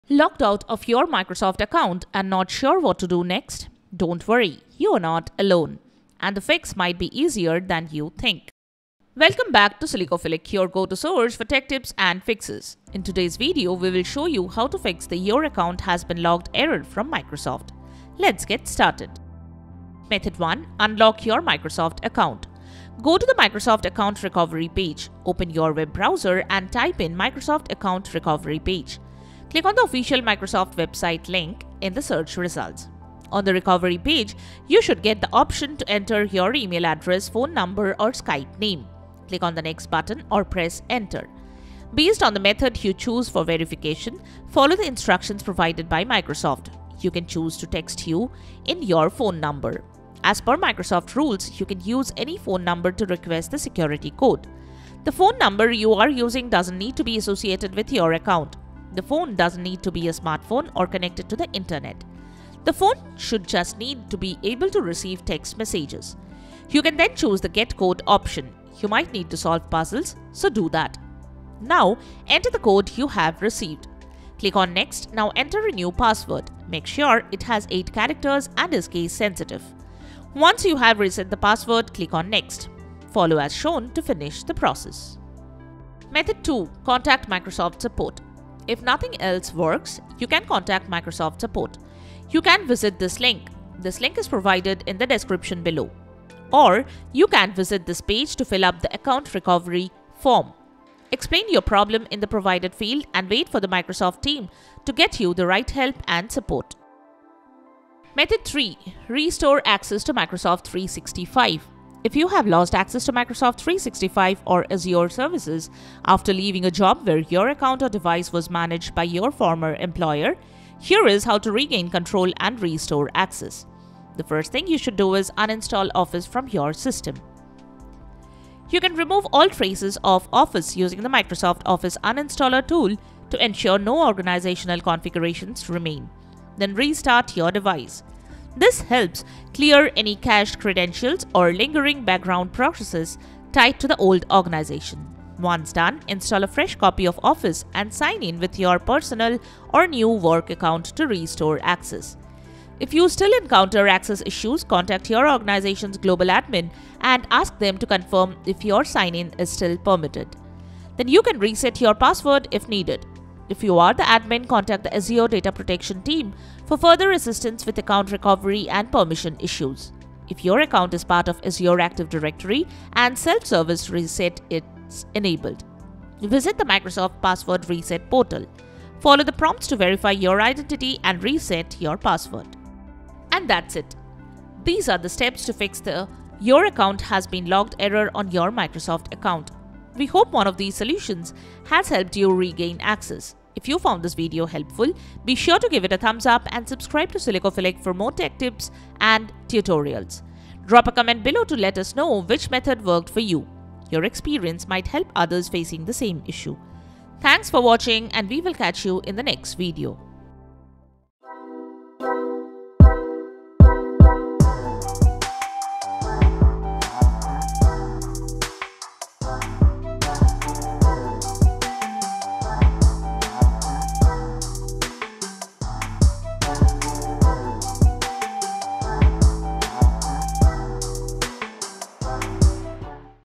How To Unlock Your Microsoft Account? | 2025
Is your Microsoft account locked and you're not sure what to do next? In this quick and easy tutorial, we’ll show you how to unlock your Microsoft account using simple steps. Whether you were locked out due to suspicious activity, too many login attempts, or security issues, we've got you covered.

Timestamps:

00:00 Intro
00:20 Method 1
01:26 Method 2
01:48 Method 3